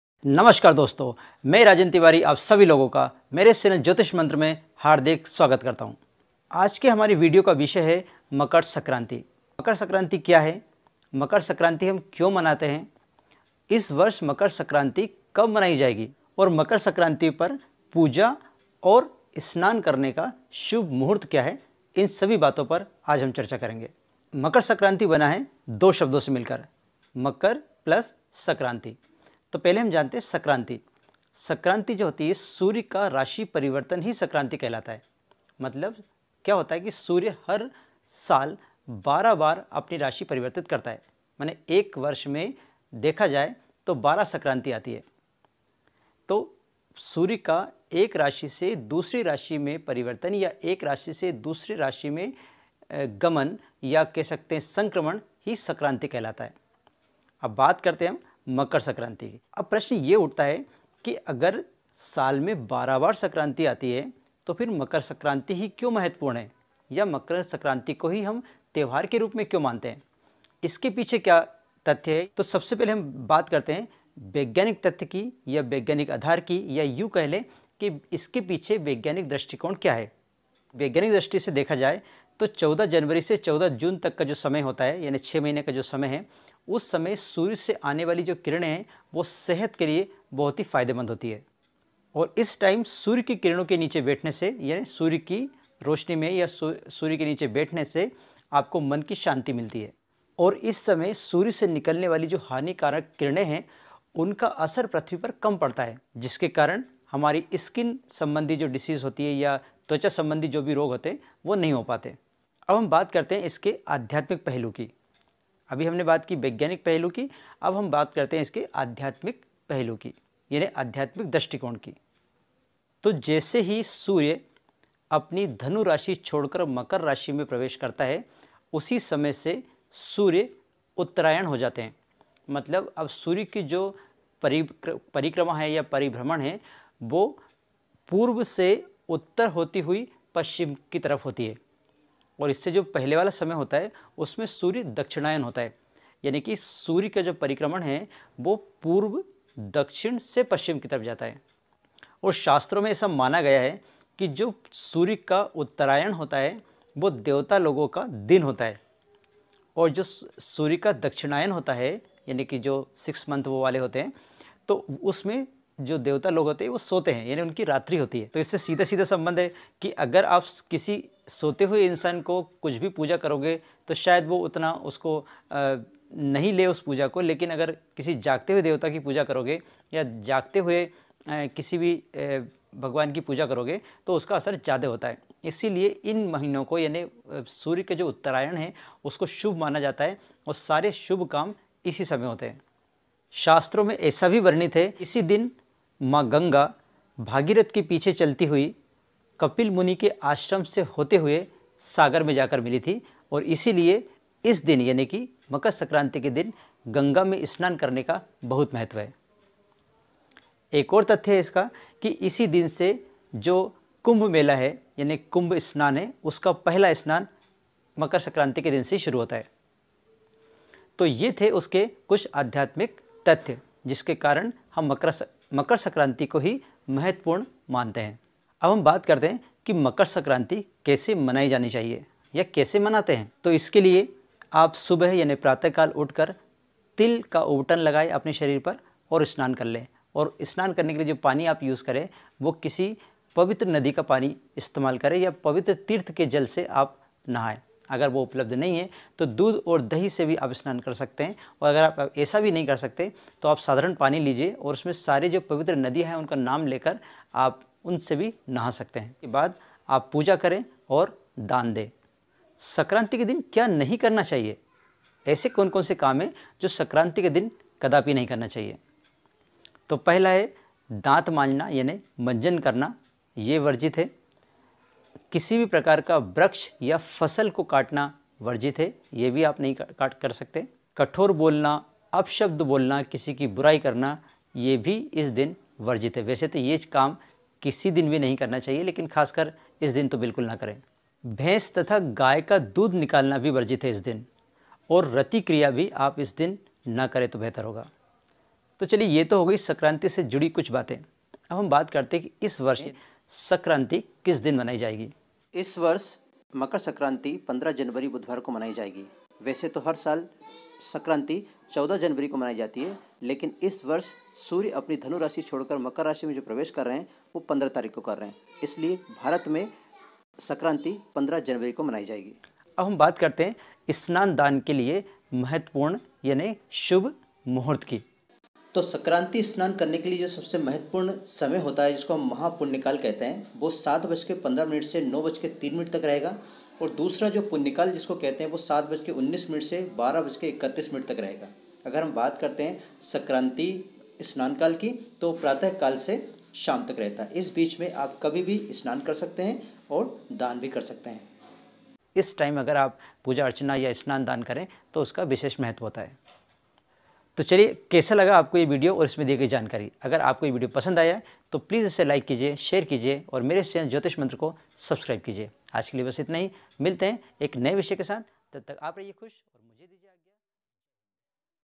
मकर सक्रांति 2020।। मकर सक्रांति कब है।।सक्रांति क्या,कब,क्यों,कैसे।।
नमस्कार तो दोस्तों आने वाली 15 जनवरी को मकर सक्रांति मनाई जाएगी । सक्रांति पर हम क्या करें, क्या ना करें, सक्रांति कब मनाई जाएगी, सक्रांति कैसे मनाई जाती है। सक्रांति को दान करने का क्या महत्व है। सक्रांति पर पूजा स्नान और दान करने का सही समय क्या है। या यूं कहे कि शुभ मुहूर्त क्या है। महा पुण्य काल, पुण्य काल, सक्रांति काल कब कब है, यह जानने के लिए आप देखते रहिए ज्योतिष मंत्र। jyotish mantraa, jyotishmantraa,jyotish mantraa by rajendra tiwari,
jyotiah mantraa